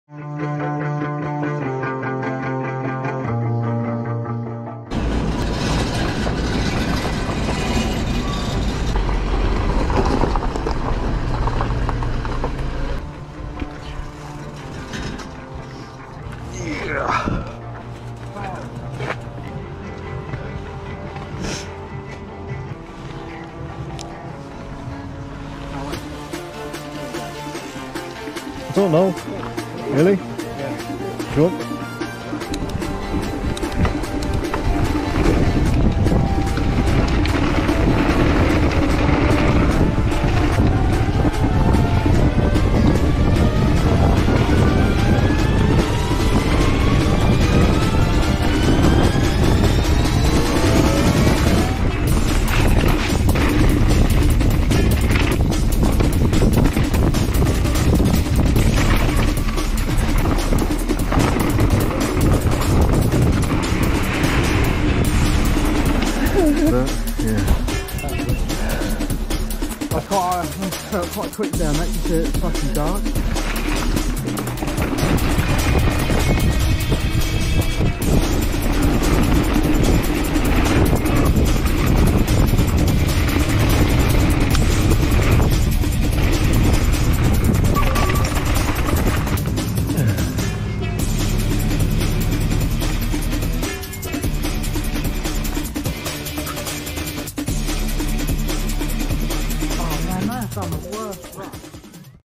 Bike park Wales night ride
Specialised kenevo night uplift ride